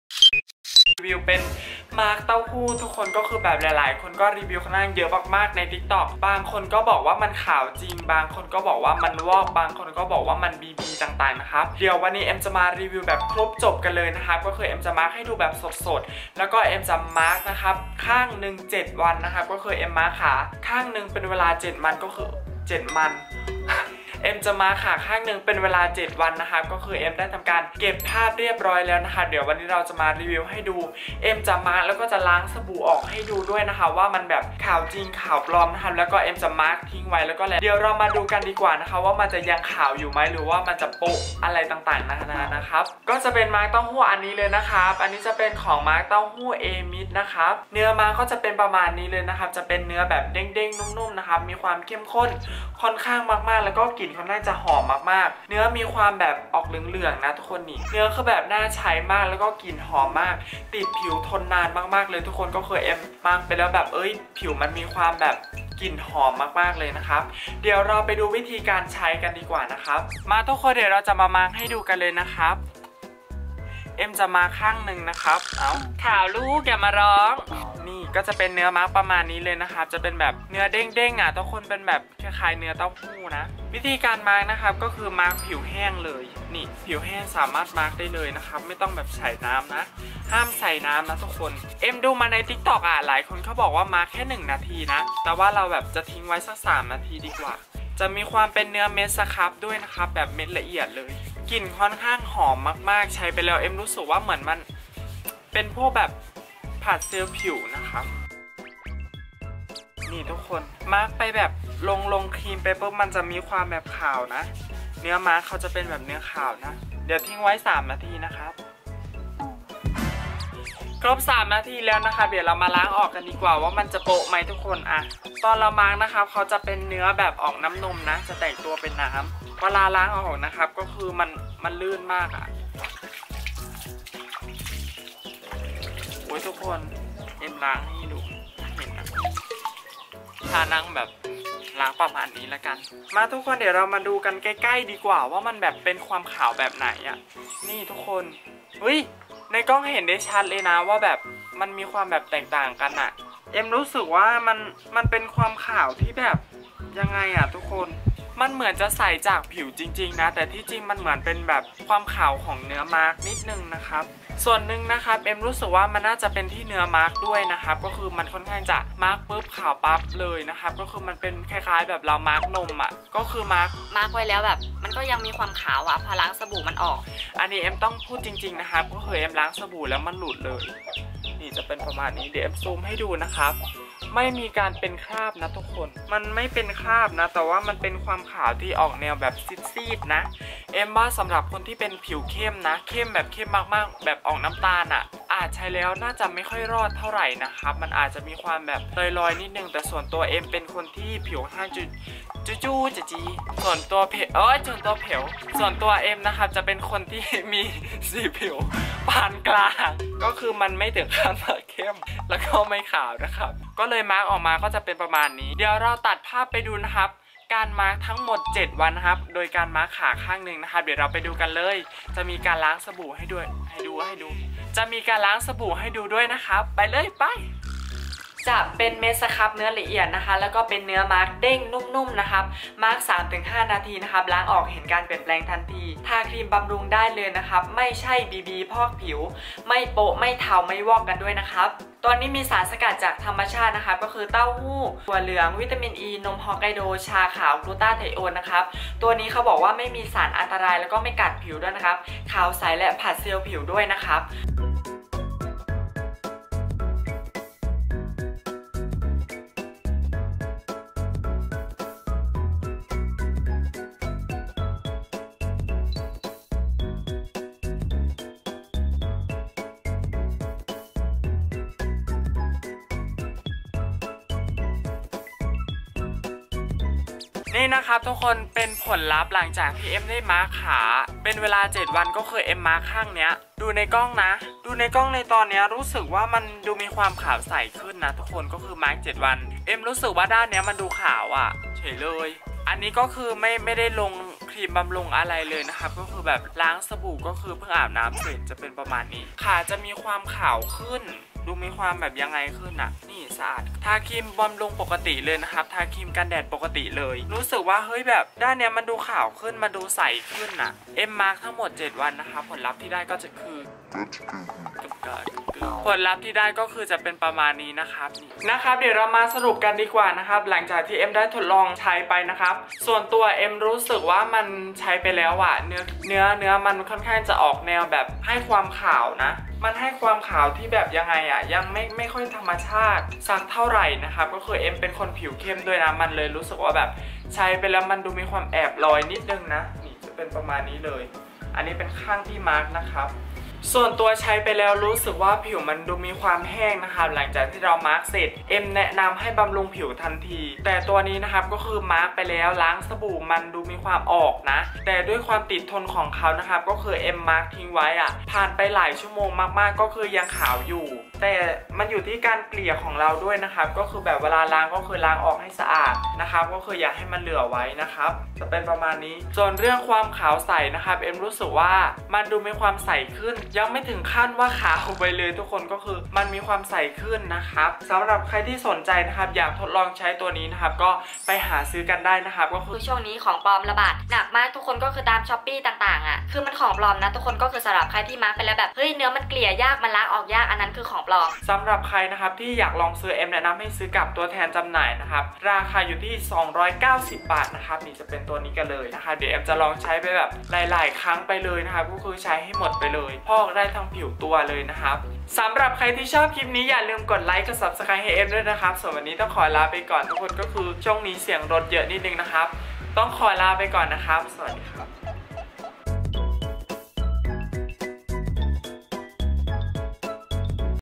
ใช้มาส์กเต้าหู้ AMICE ข้างนึ่ง 7 วัน ขาวใน 1 นาที ล้างสบู่กลับมาดำไหม ตามกระแส!!! TIKKOK
AMICE MASK TOFU GLUTA
เอมิส มาร์คเต้าหู้ กลูต้า
ราคา 290 บาท 200 g.

✔️เร่งผิวขาว
 ✔️ลดรอยแผลเป็น
 ✔️ไม่กัดผิว ไม่แสบ ไม่ทำให้ผิวบาง
 ✔️ บำรุงผิวกาย
 ✔️ช่วยให้ผิวชุ่มชื่น
 ✔️ผิวเนียนนุ่ม
 ✔️ขาวกระจ่างใส

คลิปนี้ไม่ได้จะมาโจมตีแบรนด์นะครับผลลัพธ์ต่างๆคือใช้จริง ภาพที่ออกมาคือจากการใช้ข้างนึ่ง 7 วัน ต่อเนื่อง แต่จริงใช้แค่อาทิตย์ล่ะ 1-2 ครั้งพอ ที่ว่ามันขาวคือมันก็ตามที่เห็น ส่วนตรงว่าผิวเข้มน่าจะใช้ไม่ได้ก็เป็นการ คาดไว้เฉยๆยังไงผลลัพธ์ก็อยู่ที่ผิวแต่ละบุคคลอันนี้เป็นแค่การใช้แค่ผิวเอ็ม ส่วนเรื่องขาวใสใสขึ้นจริงผิวลื่น
ส่วนตรงว่าของปลอมก็แนะนำให้ซื้อกับตัวแทนราคา290 จะดีกว่า ทางแบรนด์ไม่ได้จ้างนะ 🙏

ตอนนี้เอ็มมีเวลาลงคลิปแล้วนะครับคือ
อังคาร ศุกร์ เสาร์ เวลา 20:00

Camera : Sony a5100 เลนส์ kit